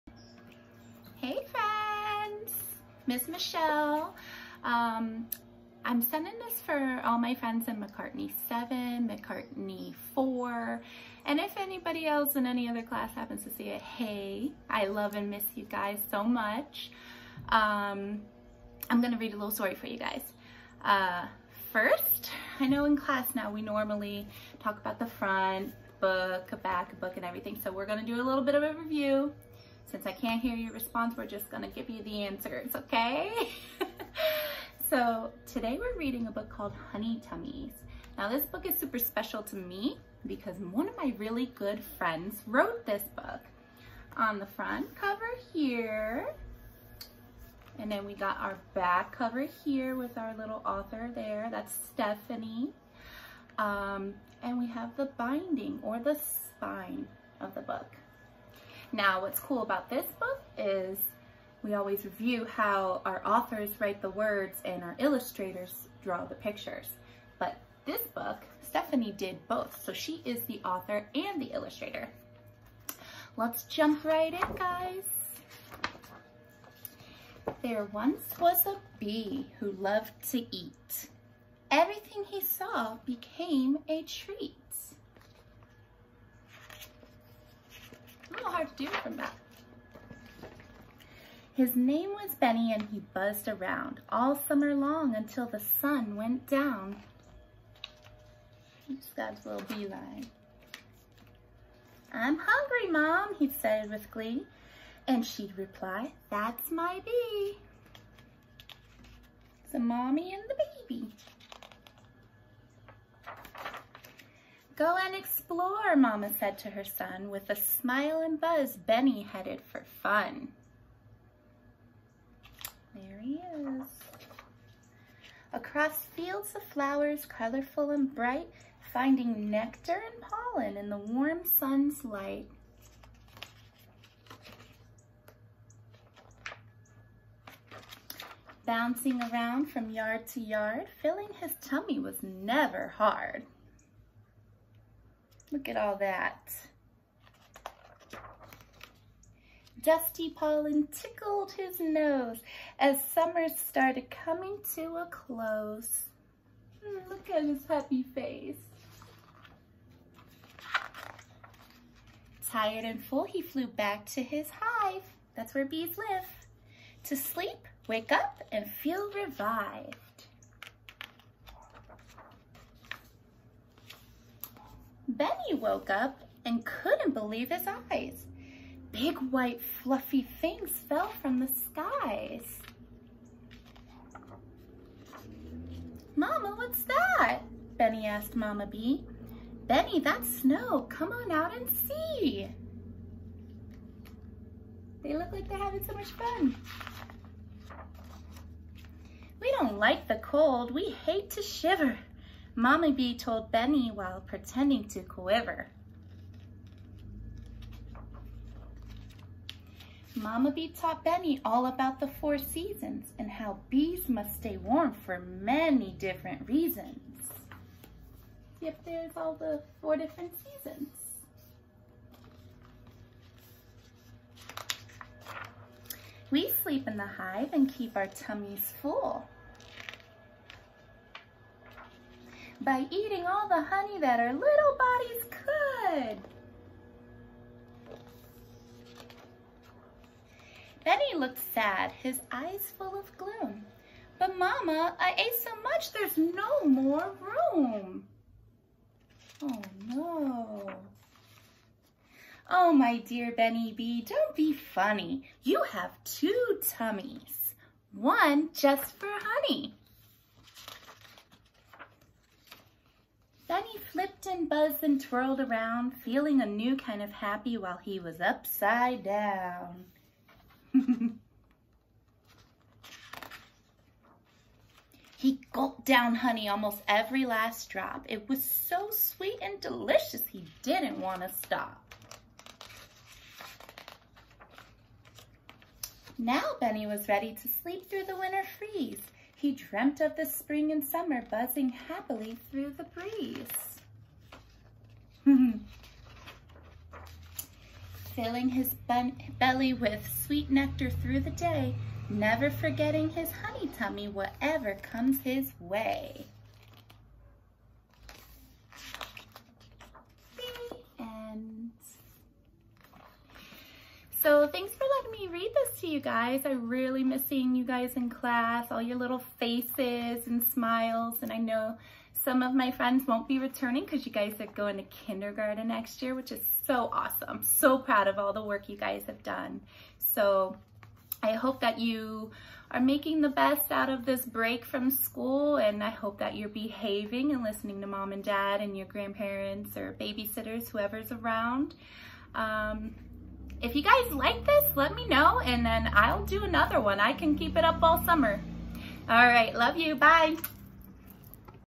Story time w/ Miss Michelle!
Honey Tummies read aloud. Written and illustrated by Stephanie Insalaco